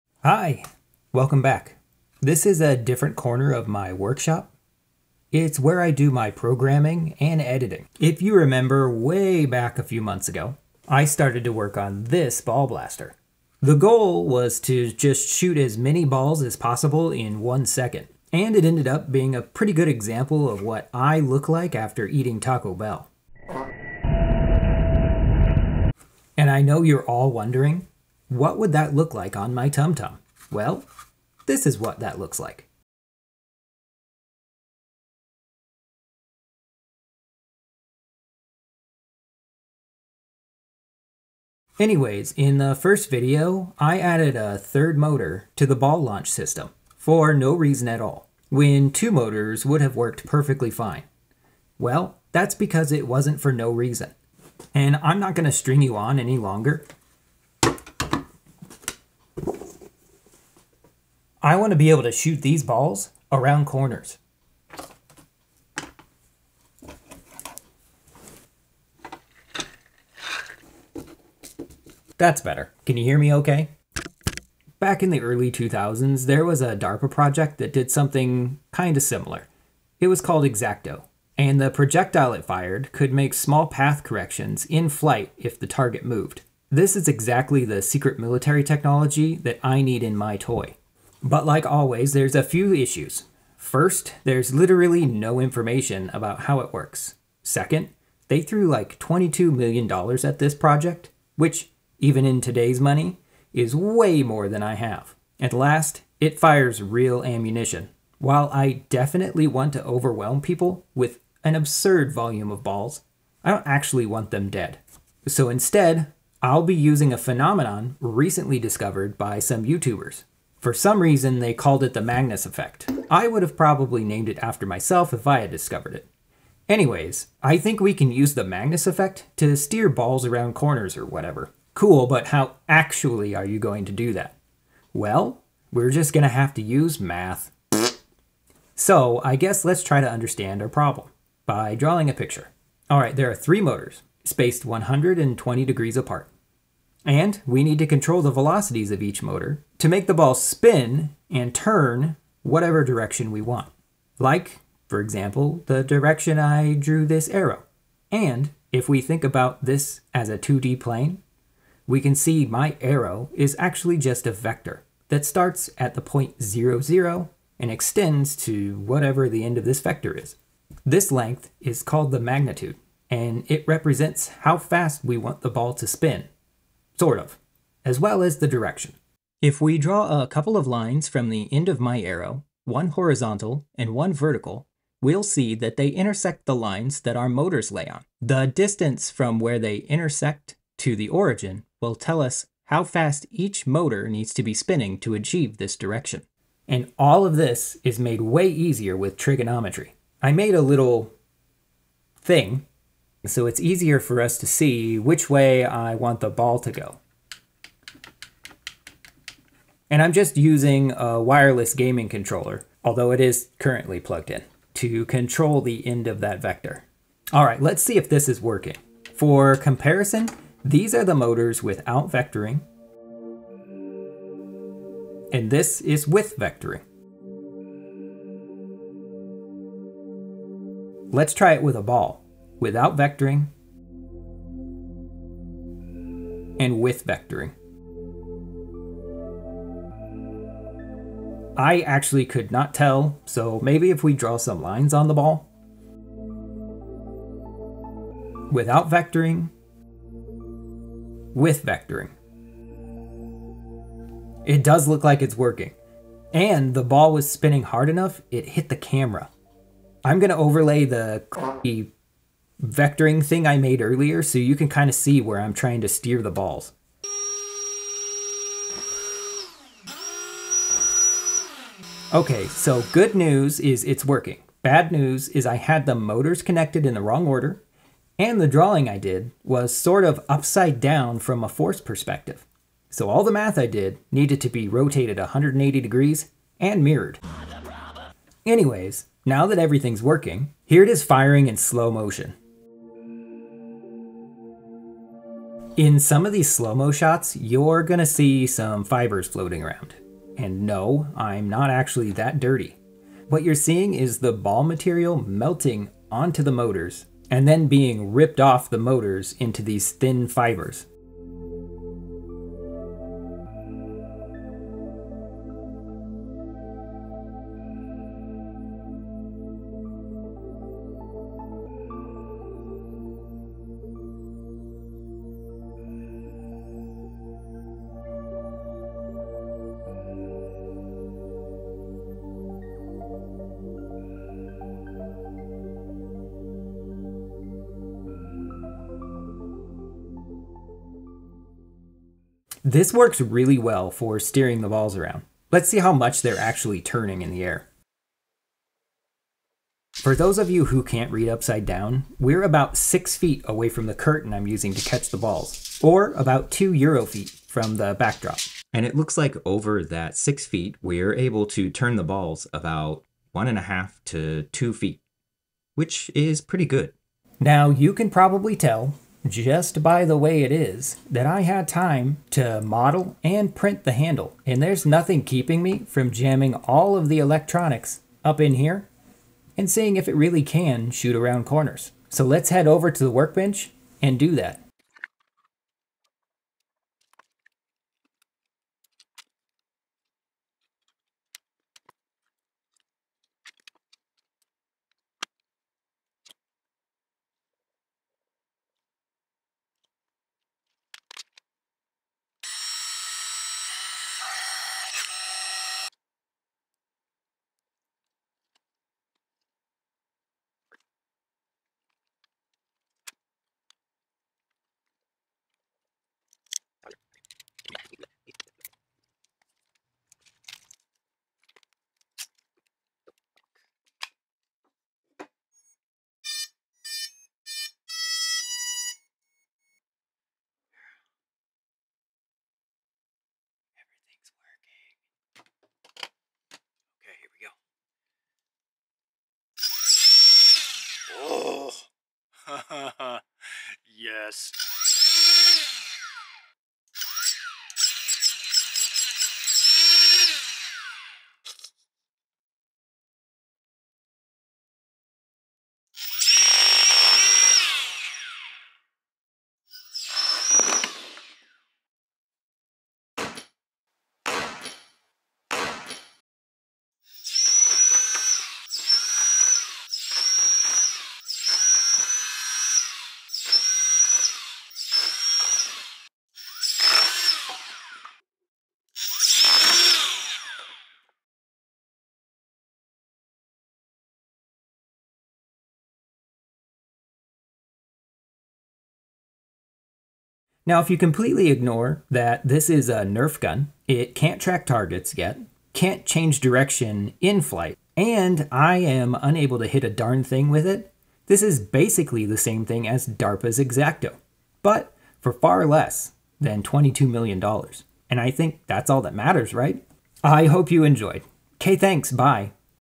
Shooting Around Corners Using the Magnus Effect
Today we struggle to shoot around corners taking inspiration from a secret DARPA project called EXACTO.

Film Equipment I Use:
Slow mo/Camera